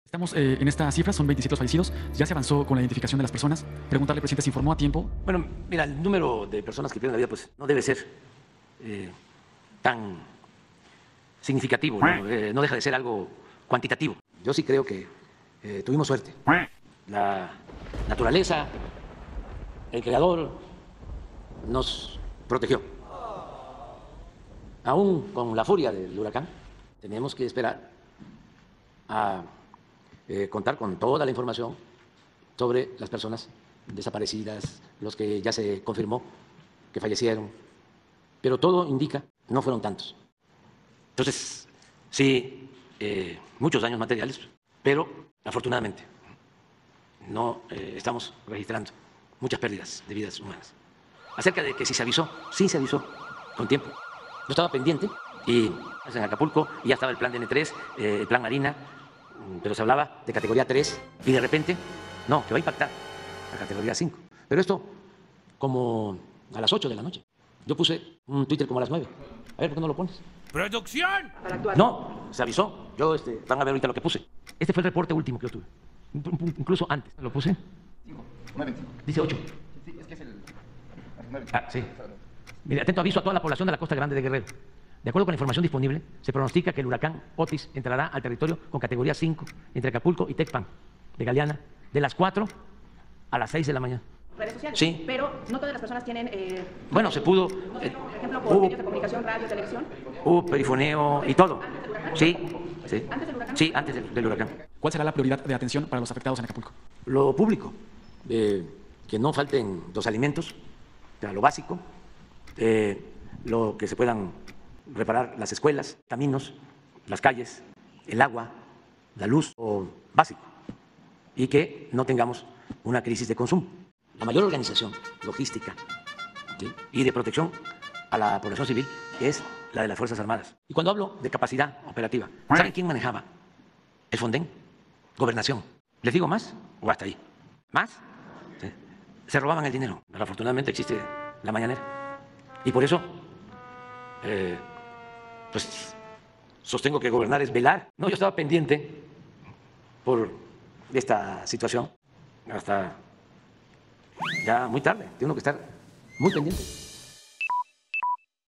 “tuvimos suerte… todo indica que no fueron tantos” fallecidos, sí se avisó a tiempo a la población🦆
Sostiene que la cifra de fallecidos por el huracán Otis es de 27, no descarta que surjan más, “pero tuvimos suerte… todo indica que no fueron tantos”, sí se avisó a tiempo, “yo puse un twitter como a las 9pm”, y asegura que él siempre estuvo pendiente 🦆